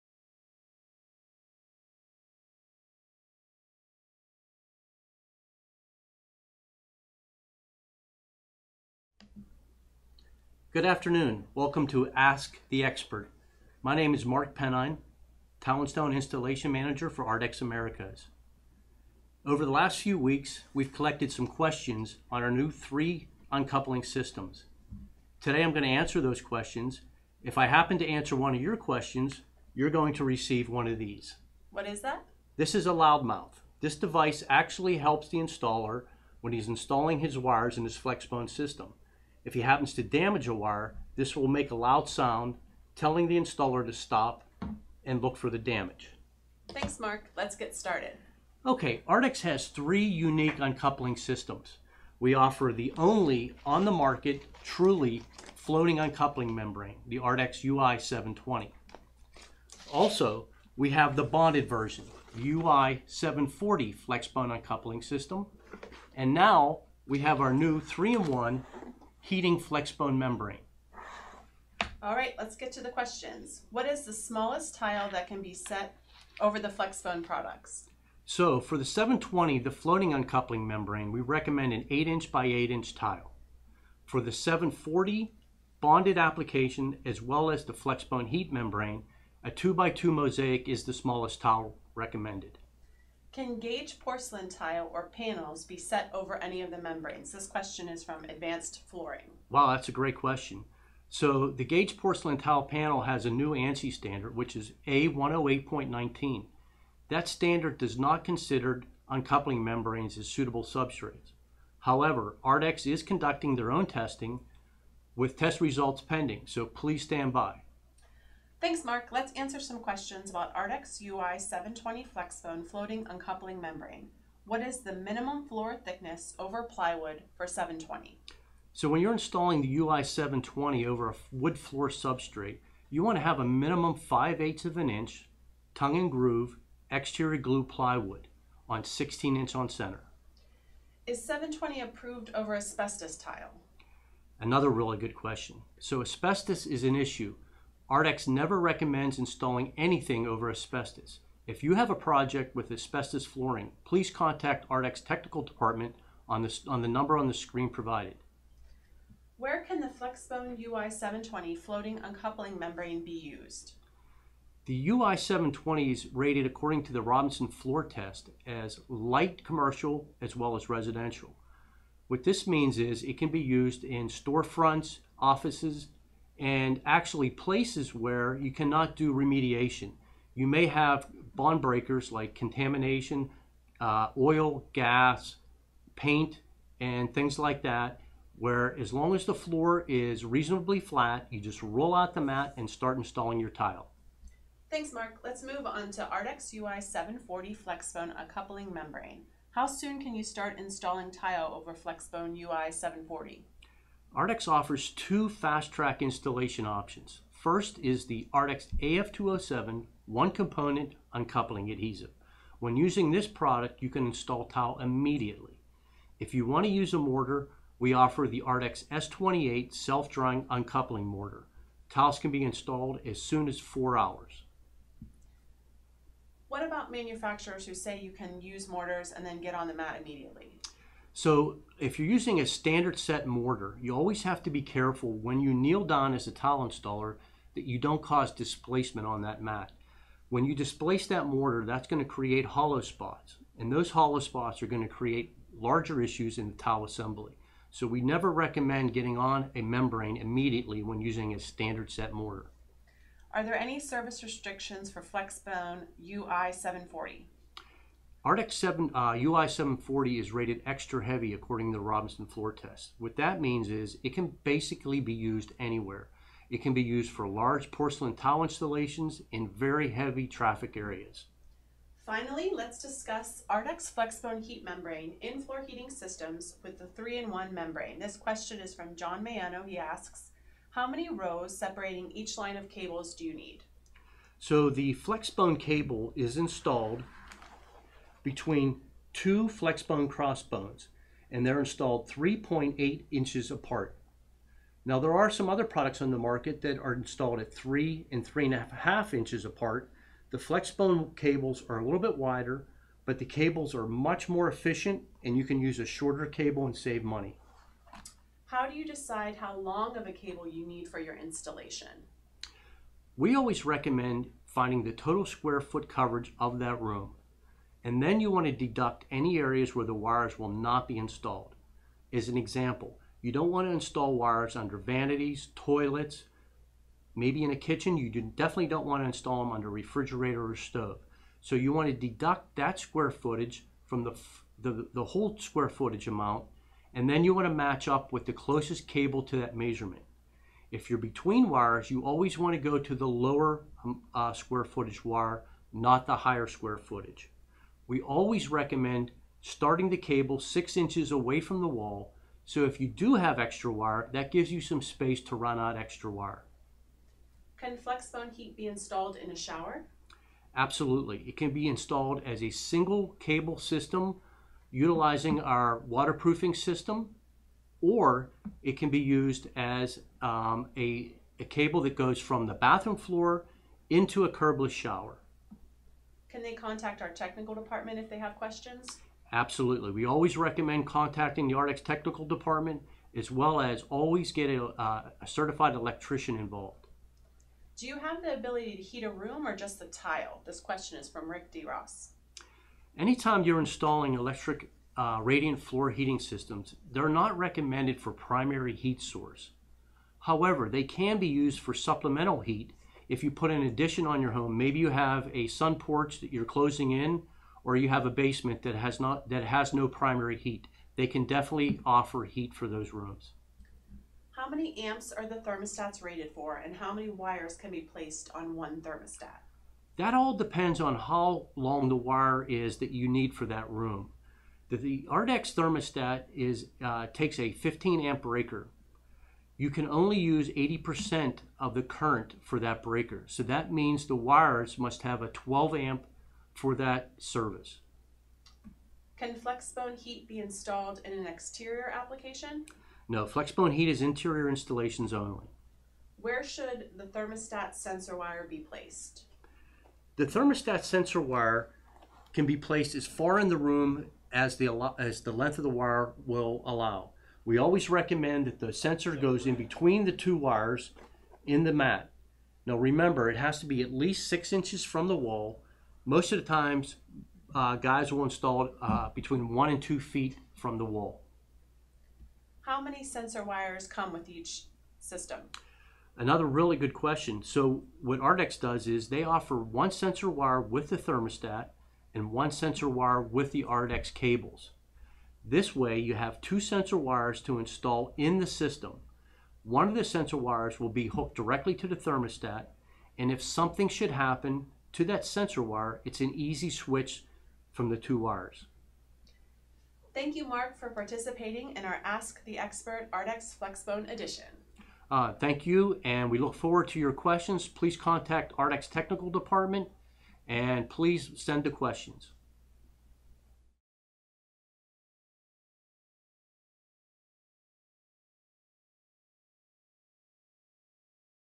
ARDEX Ask the Expert - ARDEX FLEXBONE® Edition
Check out our ARDEX Ask the Expert - ARDEX FLEXBONE® Edition video! Mark Pennine, Technical Manager - Tile & Stone, fields questions from Facebook about our ARDEX FLEXBONE UNCOUPLING AND HEATING SYSTEMS.